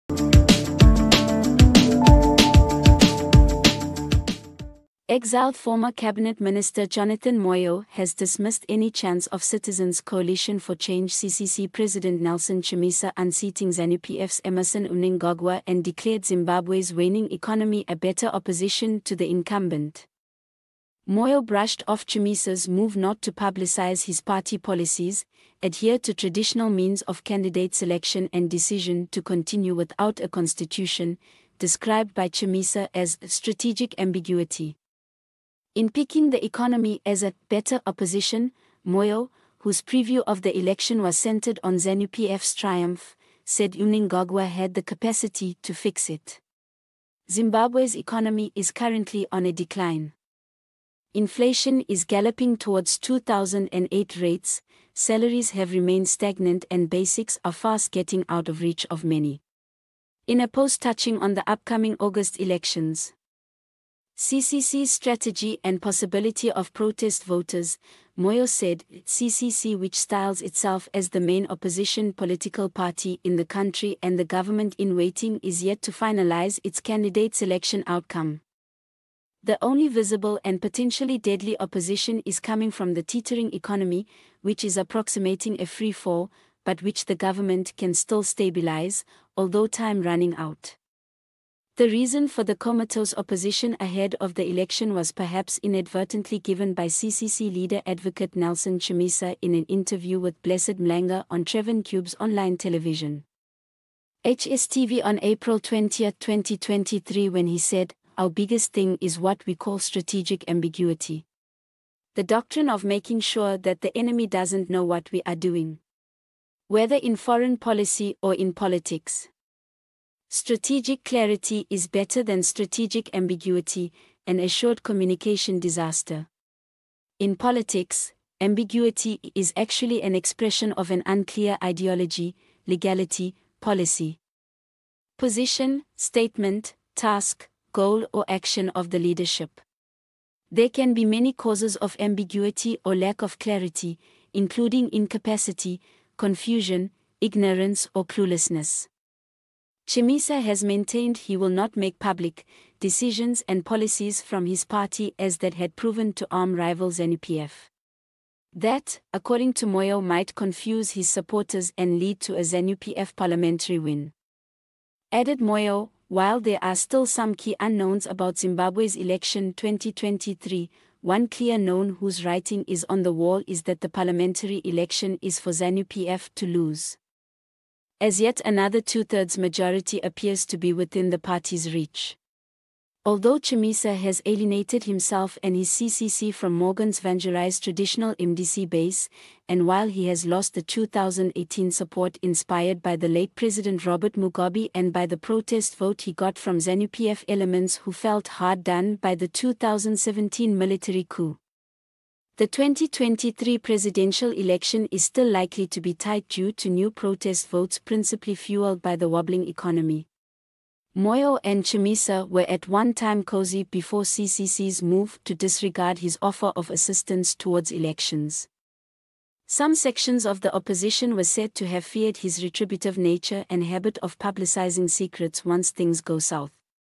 Jonathan Moyo takes a dig at Chamisa
EXILED former Cabinet Minister Jonathan Moyo has dismissed any chance of Citizens Coalition for Change (CCC) President Nelson Chamisa unseating Zanu-PF's Emmerson Mnangagwa and declared Zimbabwe's waning economy a better opposition to the incumbent.